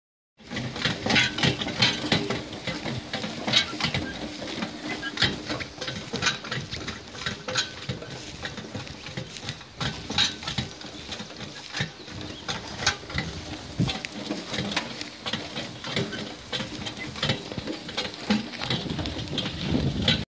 Bracken rolling at Heddon Valley
On a flat site so we can actually take a video without breaking our neck! Molly is loose which is a practical thing but also she is getting used to the roller and moving in the bracken.